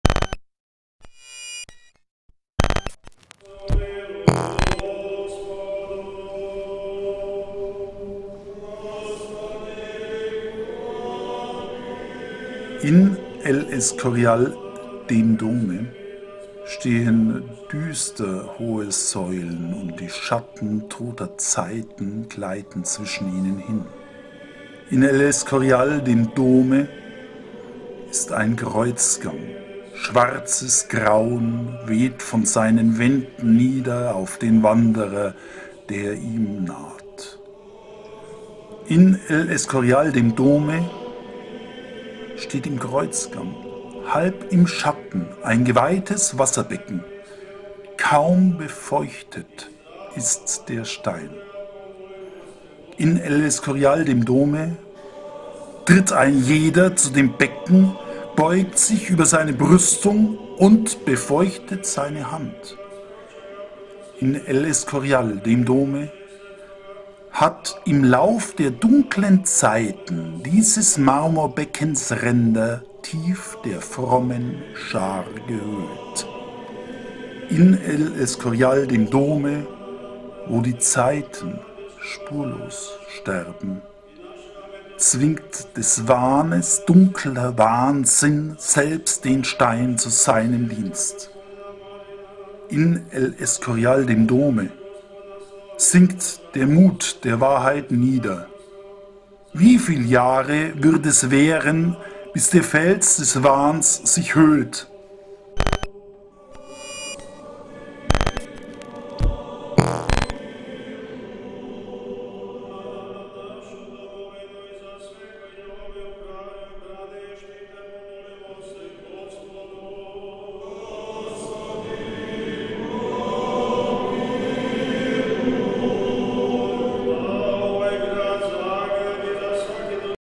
Reisepoesie Folge 7: John Henry Mackay »El Escorial«
In 21 Folgen stellt die Online-Redaktion der Zeitschrift DAS GEDICHT internationale Reisepoesie aus vier Jahrtausenden vor. So können Sie sich gemeinsam mit uns auf den Weg zur neuen Ausgabe von DAS GEDICHT begeben. Die buchstarke Nummer 21 wird ab Herbst 2013 zeitgenössische Gedichte versammeln, die sich um das Reisen drehen.

Diese Auswahl von Reisegedichten aus vier Jahrtausenden wird Ihnen von Paul-Henri Campbell präsentiert.